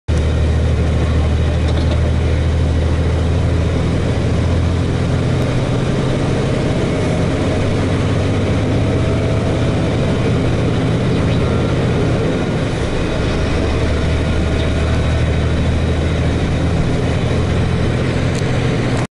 VOYAGE CAPOTOU VARIAS VEZES | 4º ECC
BR 476 Rodovia do Xisto alça de acesso Anel Viário Contorno Sul de Curitiba-PR